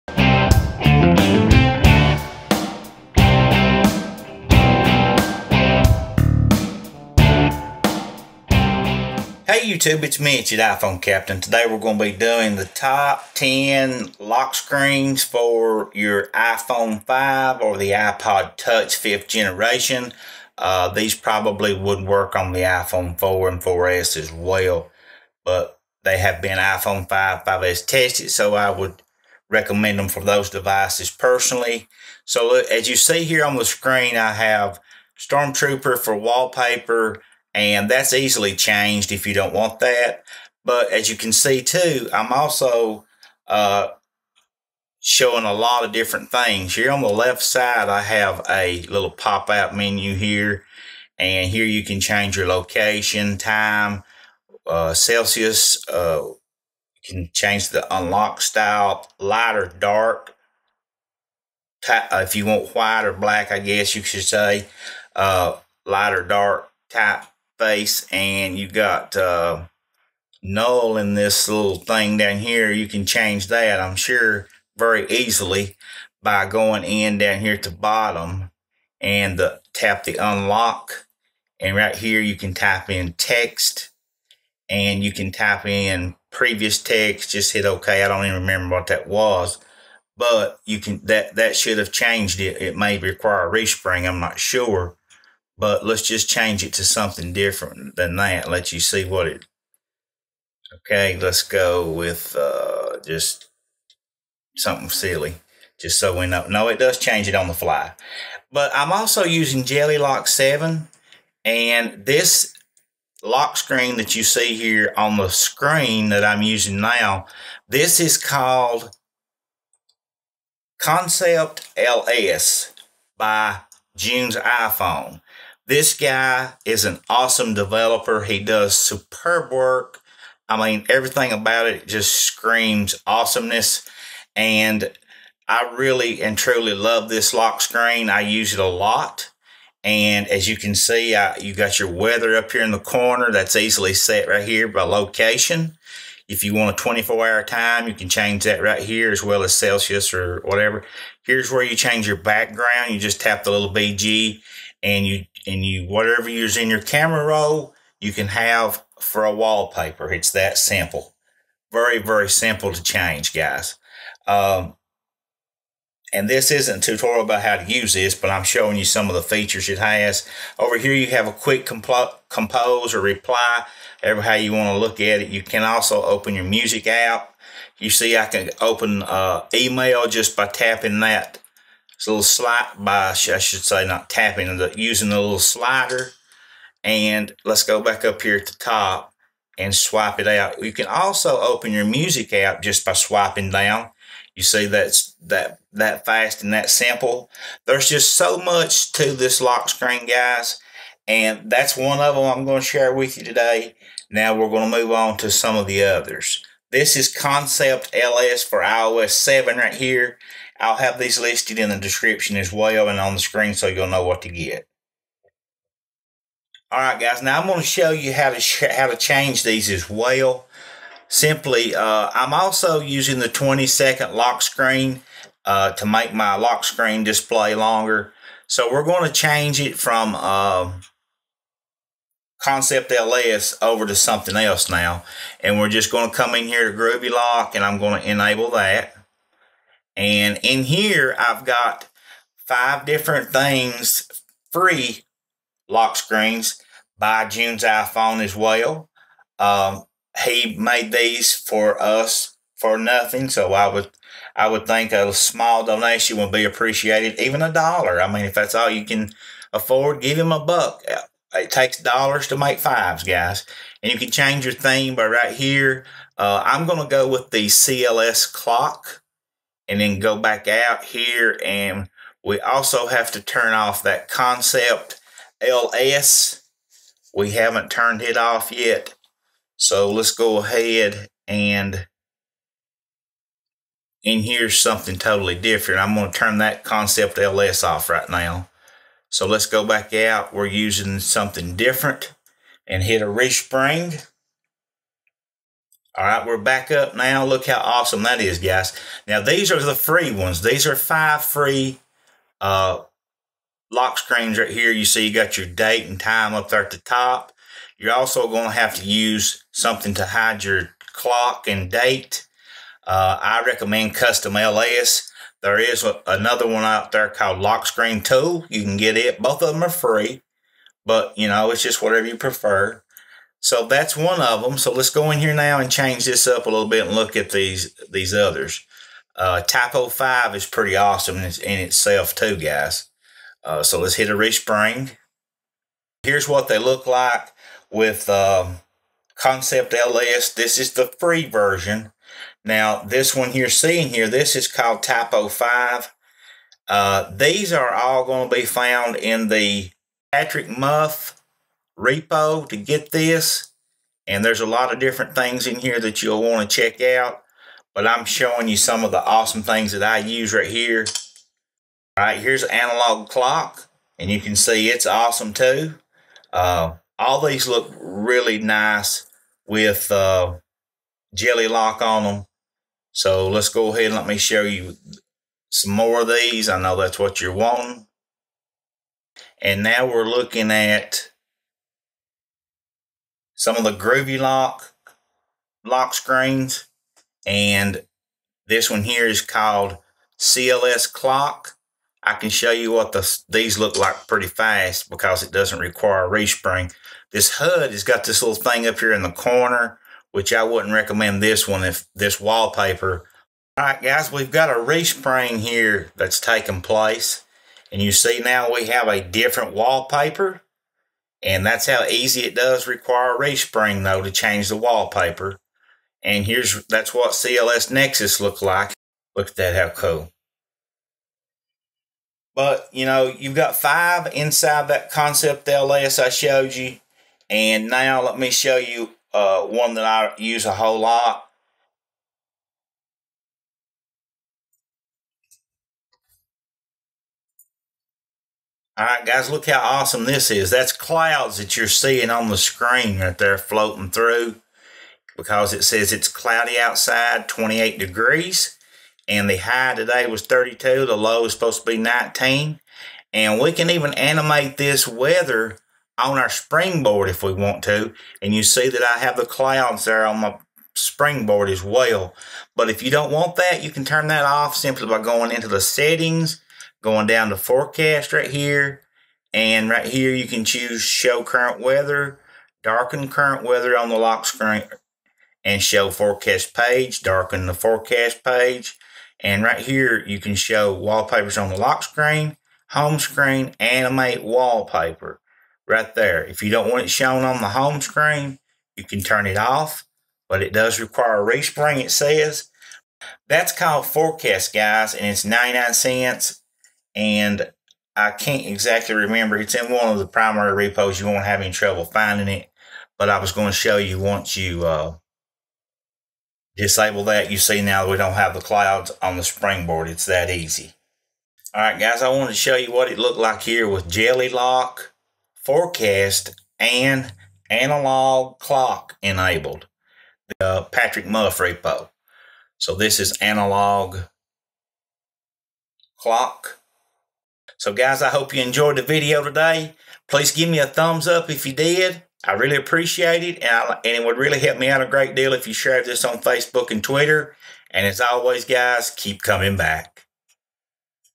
Top 10 Best Lockscreen Theme Tweaks Febraury 2014 iPhone 5/5S/5C Evasi0n7 Jailbreak 2014
Top 10 Best Lockscreens Febraury 2014 iPhone 5/5S/5C Evasi0n7 Jailbreak 2014

ConceptLS iOS 7
ConceptLS Lockscreen Pack
CustomLS
20 Second Lockscreen
Cydget
GroovyLock
iOS 7 analogClock Cydget
iOS 7 Typo5 Cydget
JellyLock7
Forecast

SOCIAL US